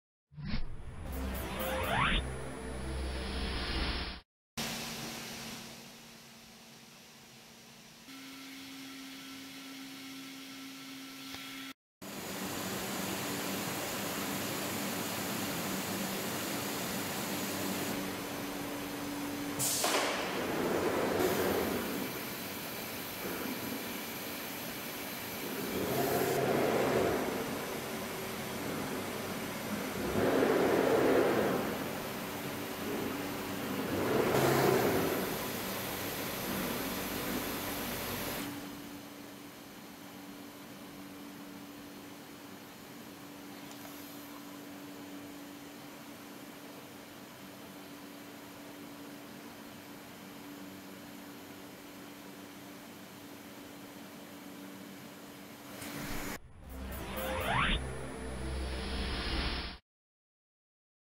AXYZ AVS Camera option
A high performance, low-cost solution suitable for use on a variety of CNC applications. The power behind the AVS lies in its software which is fully integrated with the A2MC Control system. The digital print finishing benefits from the ability to quickly find and recognize conventional registration marks and adjust the machine cutting path as necessary to achieve exact alignment with printed shapes. The AVS is compatible with all cutting tools and allows virtually any printed material to be cut including card, vinyl, rubber, foam, aluminum, wood, acrylic and many more.